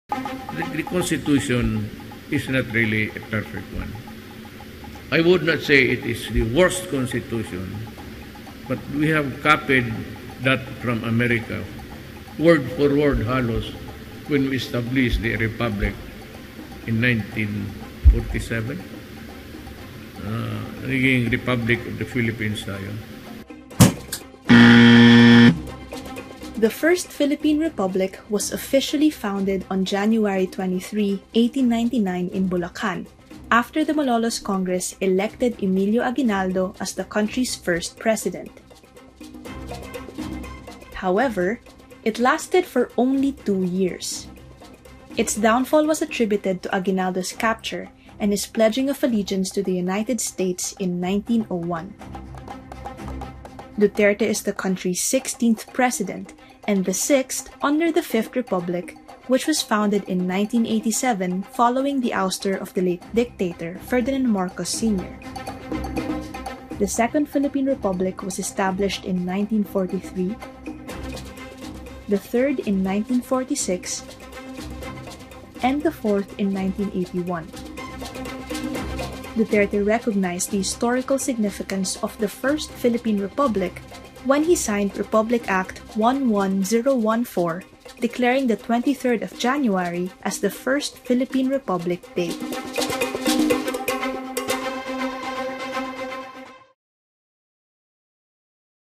VERAfied: Duterte spews false claims about Philippine republic, Durano political clan
Rodrigo Duterte, the 16th president of the Philippines, erred in citing the year of establishment of the country's first republic during an April 4 public speech in Mataasnakahoy, Batangas.

Copyright advisory:
This material is protected by copyright. You may not copy, reproduce or distribute any part of this material without  the expressed written consent of Vera Files.